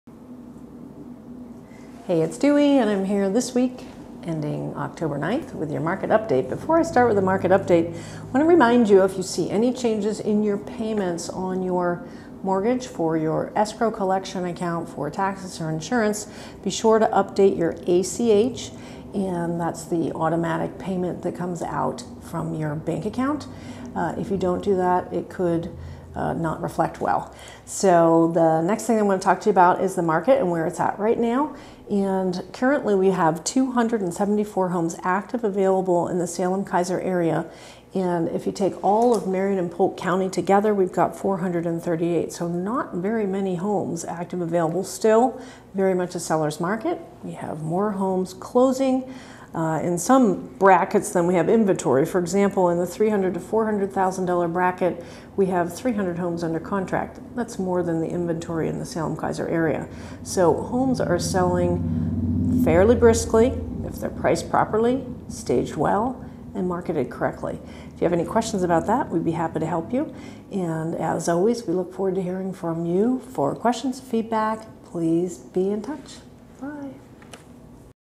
Weekly Market Update with Pro-Tip
Weekly market update for the week ending 10/9/2020.  We are still very much in a sellers market!  To find out how this affects you, drop us a line!

Friendly Reminder: Don’t forget to update your recurring payments if your taxes and insurance change this year.  Don’t run the risk of a late payment for something so simple!